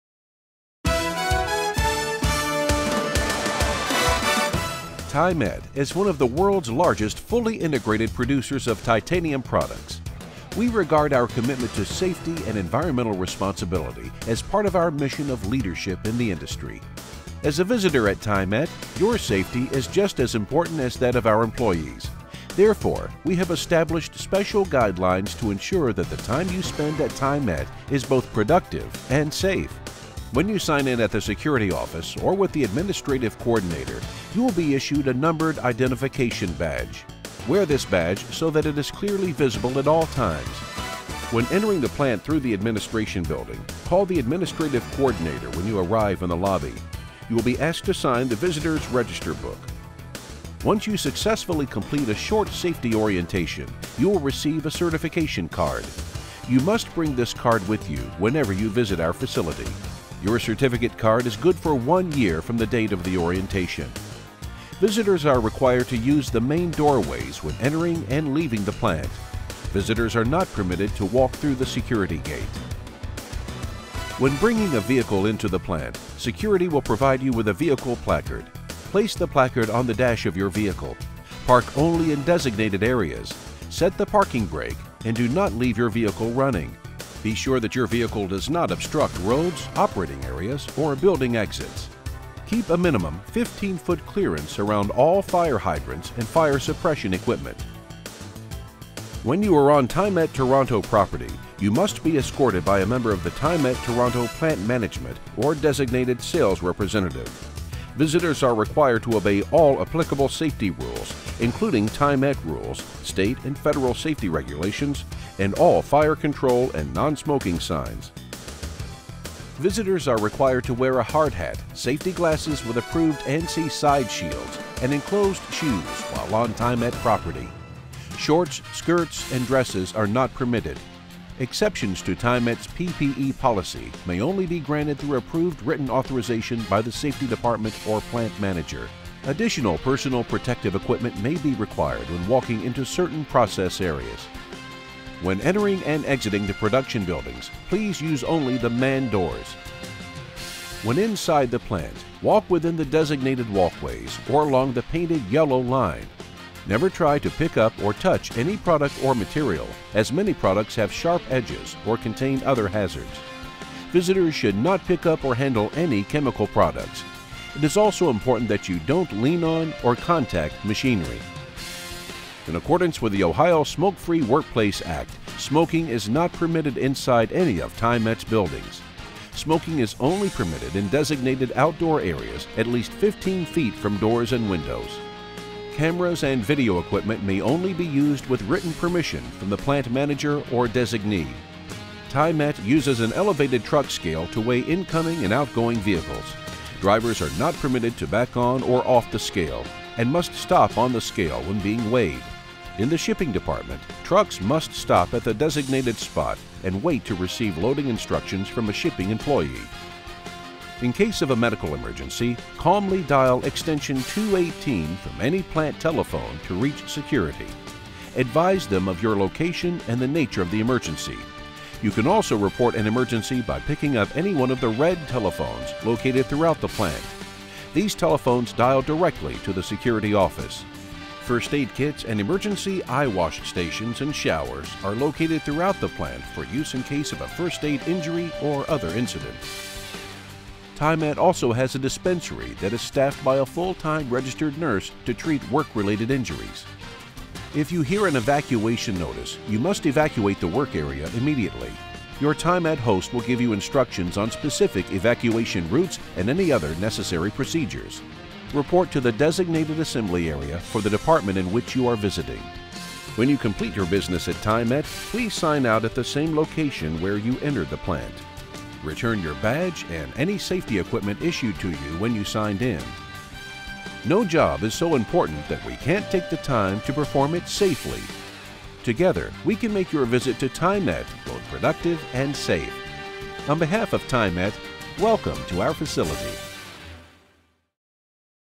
TIMET Corp
Safety video for TIMET Corporation. Since 1950, TIMET has been leading the industry in mill and melted products, supplying nearly one-fifth of the world’s titanium demand. They are the only supplier with production facilities in both the United States and Europe to support our multinational customer base.